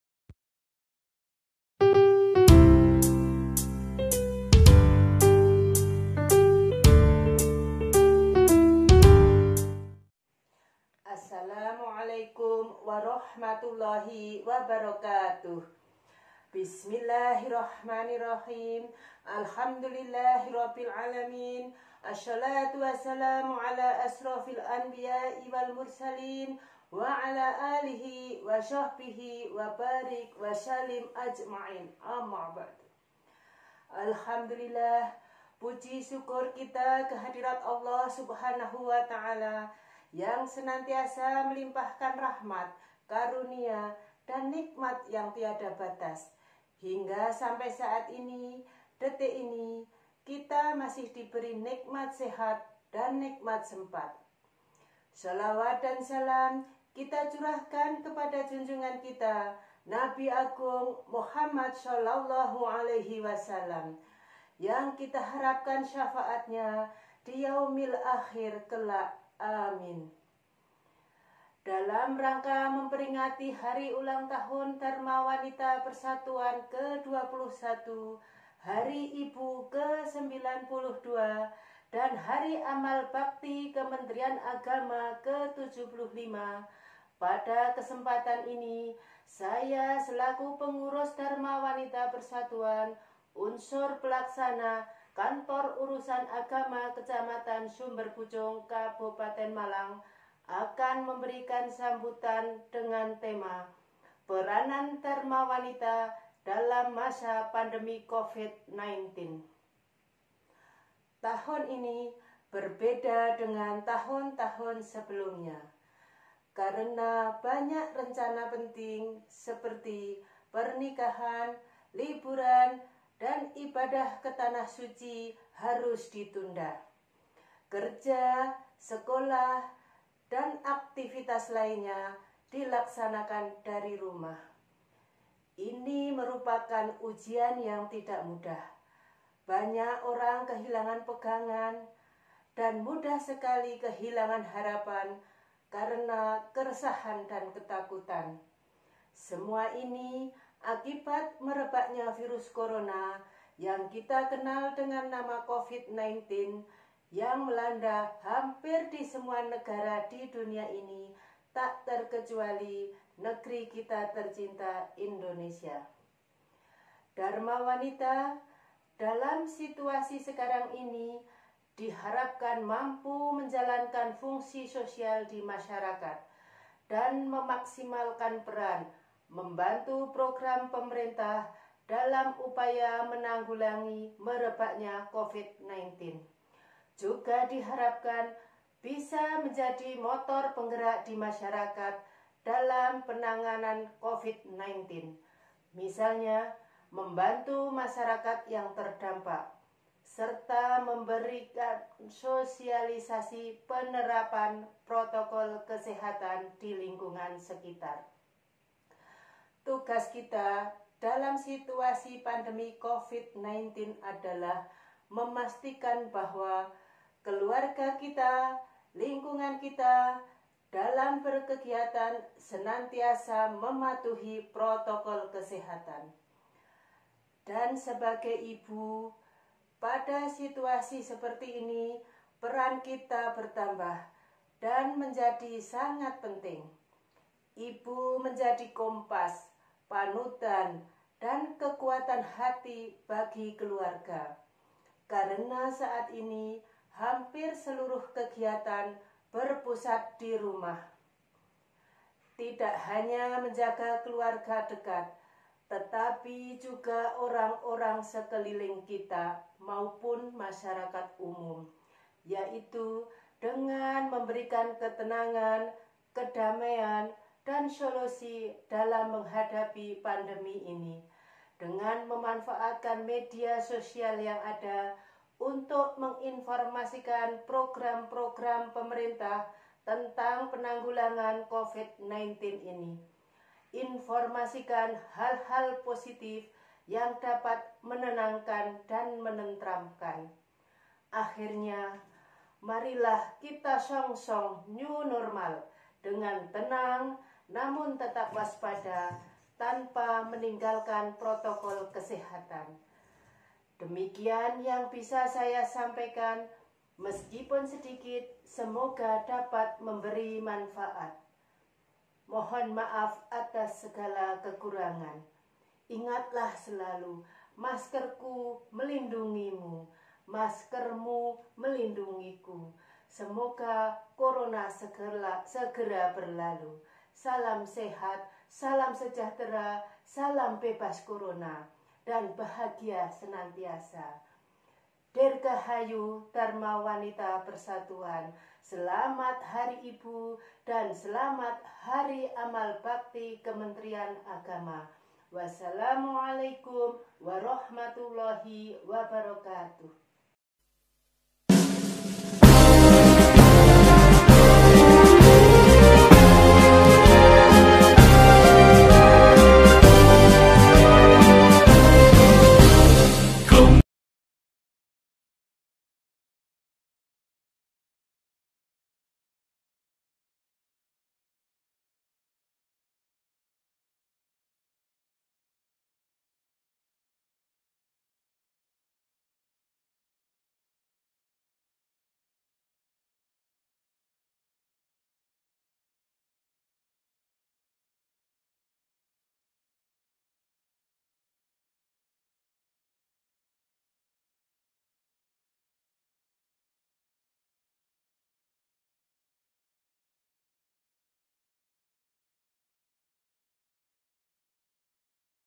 24. Peran Dharma Wanita Persatuan di Era Pandemi COVID-19 oleh Ibu Hanif Asriadi (KUA Sumberpucung)
Reupload lomba pidato virtual DWP Kemenag Kab. Malang dalam rangka HUT DWP Ke-21, Hari Ibu Ke-92 dan HAB Kemenag RI Ke-75.